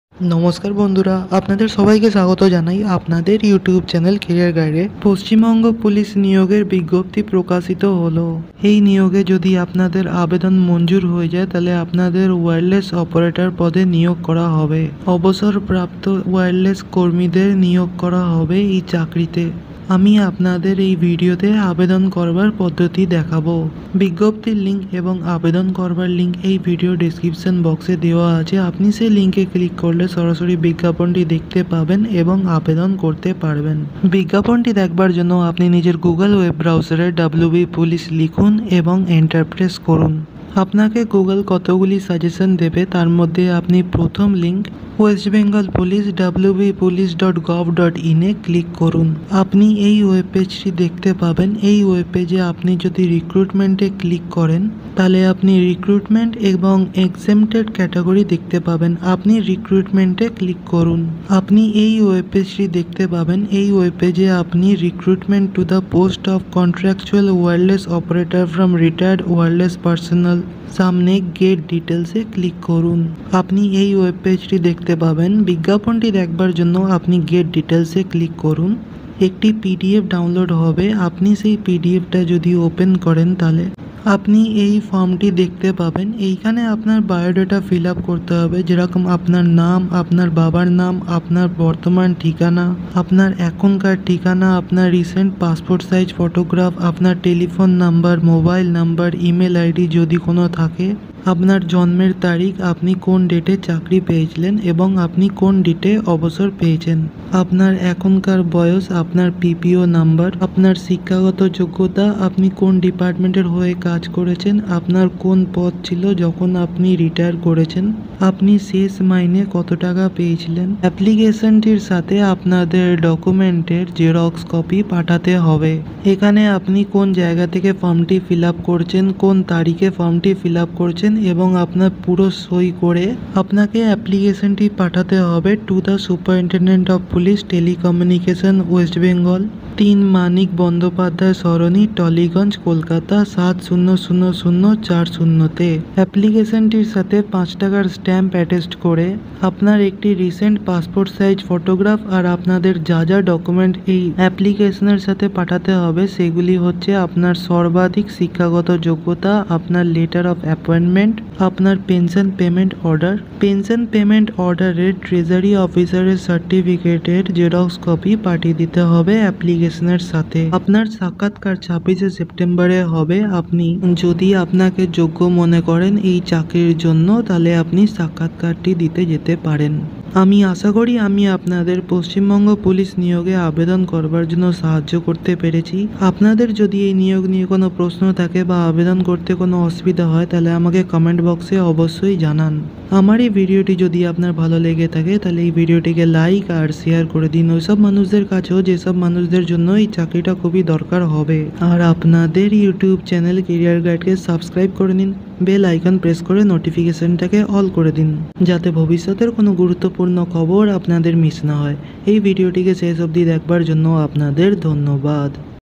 পশ্চিমবঙ্গ পুলিশ নিয়োগের বিজ্ঞপ্তি
পশ্চিমবঙ্গ পুলিশ নিয়োগের বিজ্ঞপ্তি ওয়্যারলেস অপারেটর পদে নিয়োগ অবসরপ্রাপ্ত ওয়্যারলেস কর্মীরা আবেদন করুন অফলিনে আবেদন|